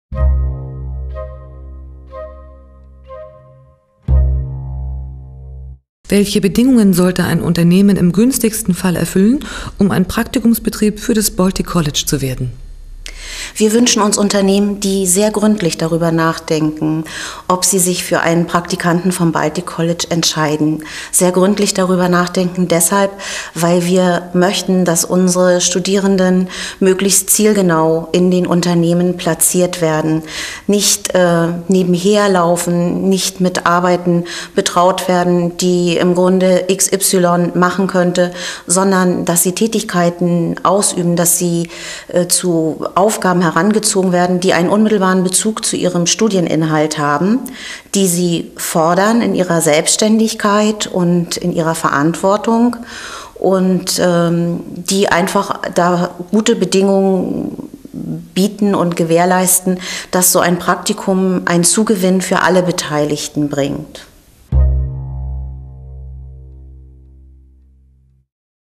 Baltic College - Praktikumsunternehmen
Welche Bedingungen sollte ein Unternehmen im günstigsten Fall erfüllen, um ein Praktikumsbetrieb für das Baltic College zu werden?